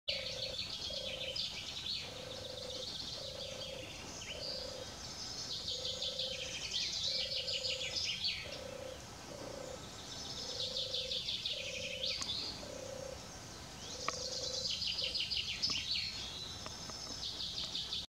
Zomertortel 13-06-21
Roepende Zomertortel op grote afstand in de buurt van Grolloo